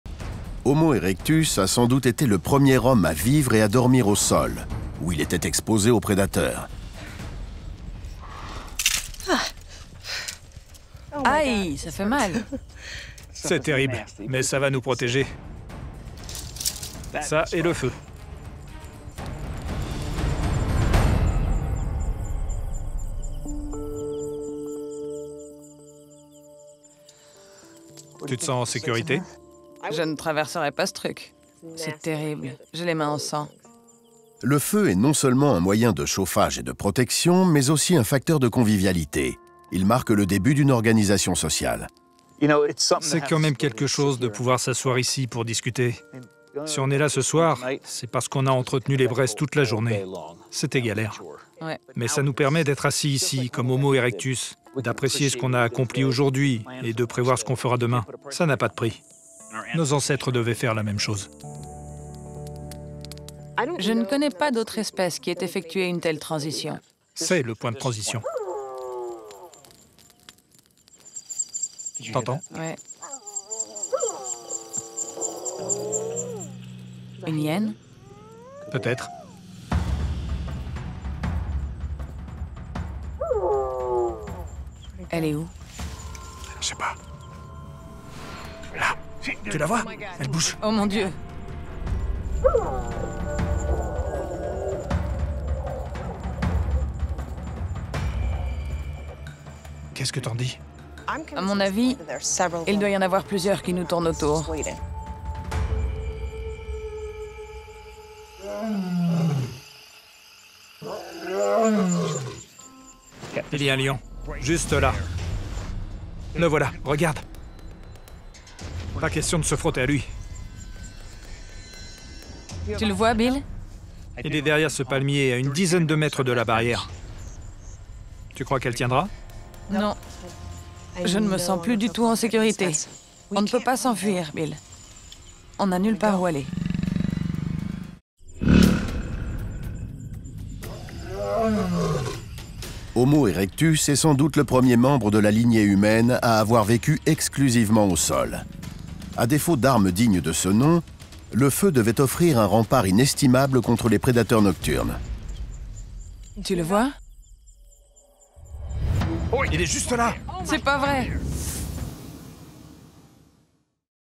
Comment le feu a permis à l'Homo erectus de dormir au sol
L'Homo erectus, en vivant au sol, était particulièrement exposé aux prédateurs. Il lui a fallu trouver des stratégies et maîtriser le feu pour assurer sa sécurité.

National Geographic, disponible avec Canal +, canal 115.